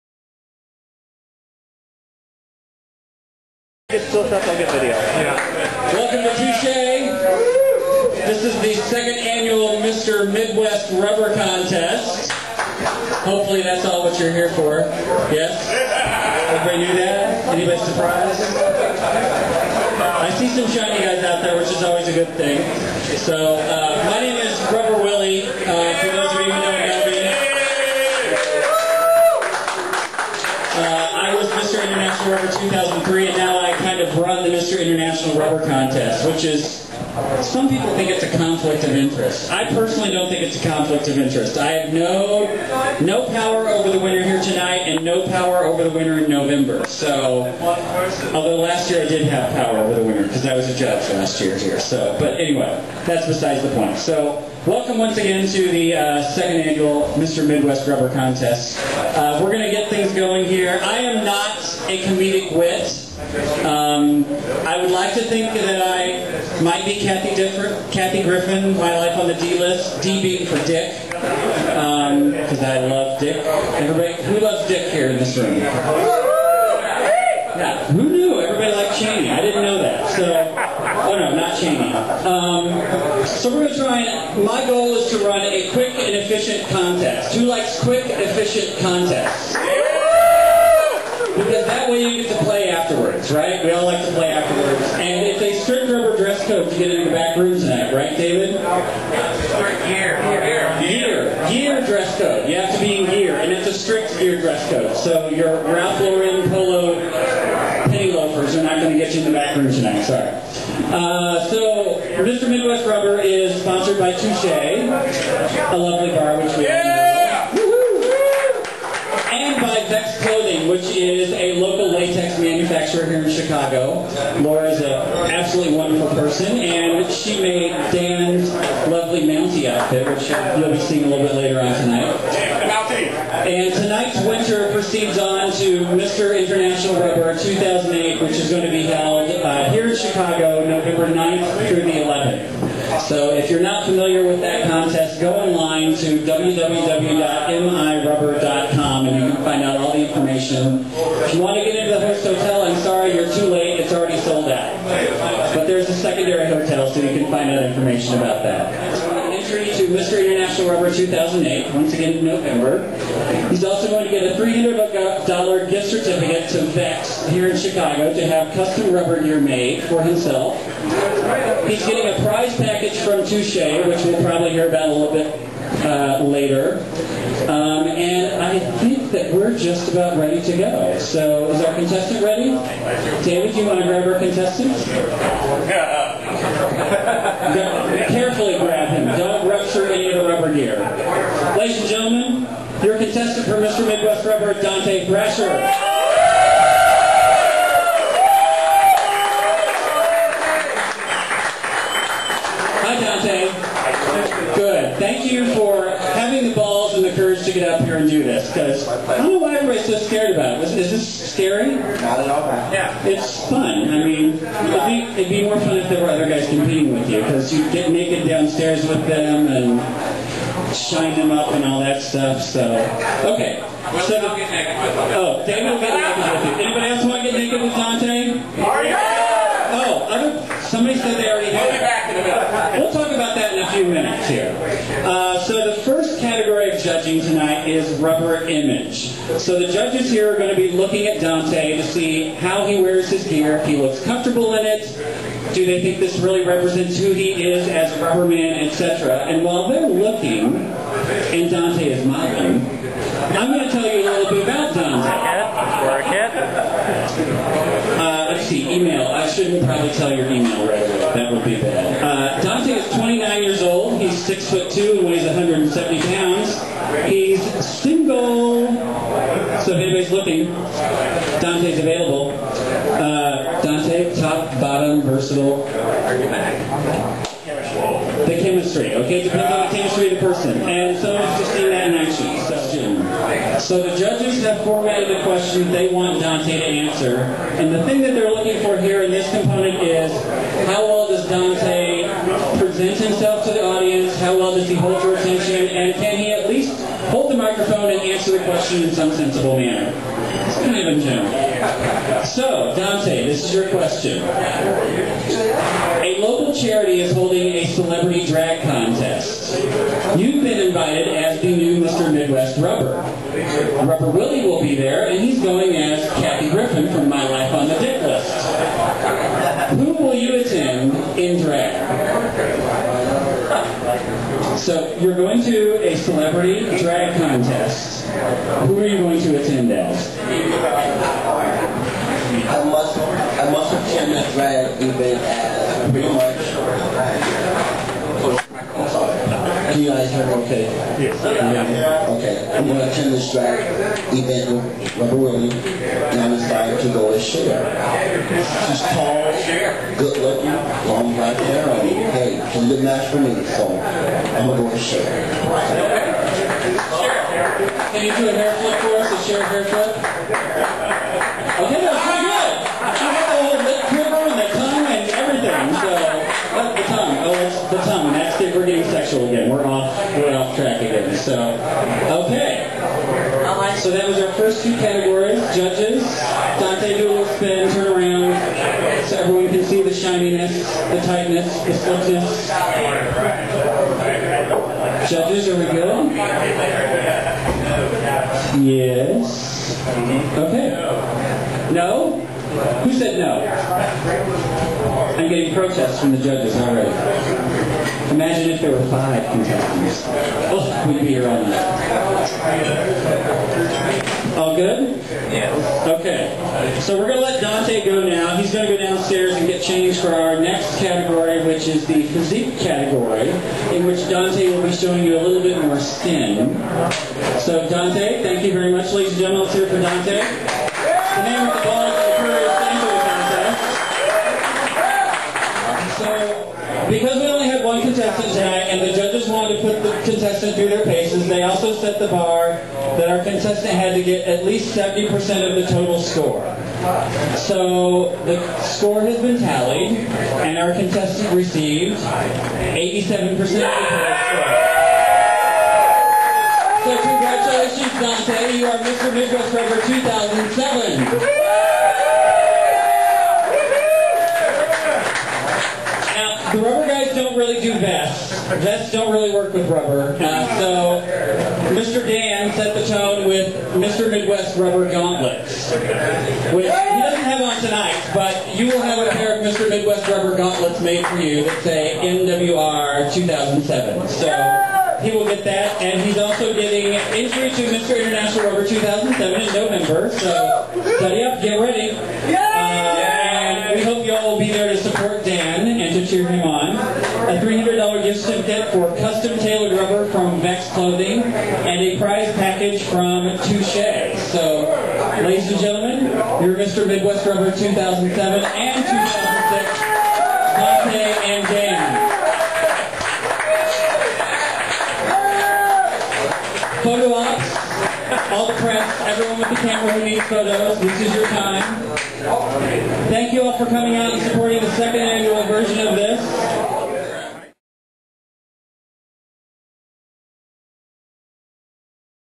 Chicago Den Video - Mr Midwest Rubber 2007
The Chicago Den's video coverage of the 2007 Mr. Midwest Rubber Contest.